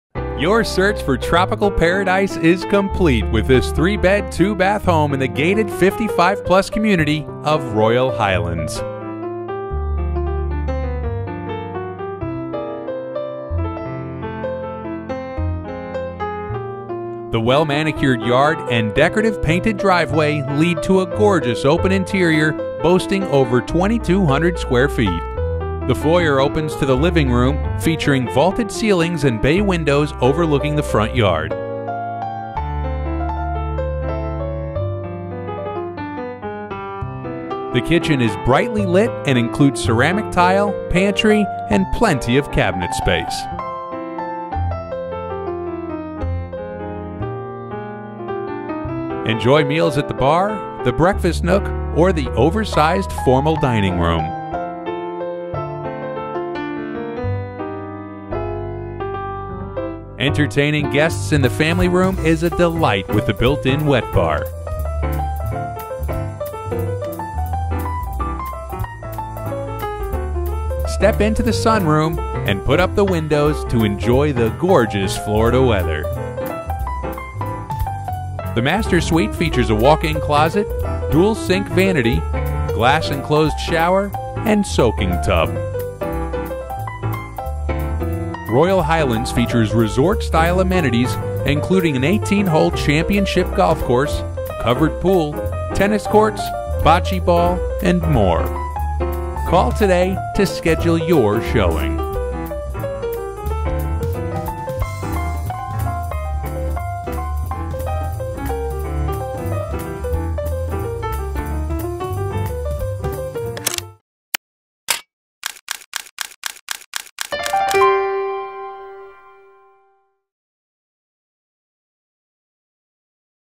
5415 Queen Victoria Drive, Royal Highlands
If you are looking for the perfect home in Royal Highlands golf community, here it is. This bright and immaculate split plan 3 bedroom, 2 bath home is built with POLYSTEEL construction for safety and insulation. It is expanded with an enclosed front porch area, bonus enclosed Florida room, as well as a covered lanai for enjoying breezy afternoons. Everything is new! New carpet, new roof, new A/C, newer refrigerator , as well as freshly painted inside and out. The home is meticulously cared for, no pets, no smoking—full of extra storage, and pocket doors for easy privacy. There is an oversized garage with extra windows, side door, peg board for tools, and ceiling fan: perfect for hobbies and fits your golf cart! Painted drive and walk way. It is an easy walk to the many amenities in Royal Highlands. Royal Highlands includes 24-hour manned gate, two clubhouses, indoor and outdoor pool, card room, arts and crafts room, library, billiards, grand ballroom, softball, tennis, bocce ball, horseshoes, picnic areas and lakefront boardwalk. The 18-hole par-72 championship golf course features a restaurant and bar.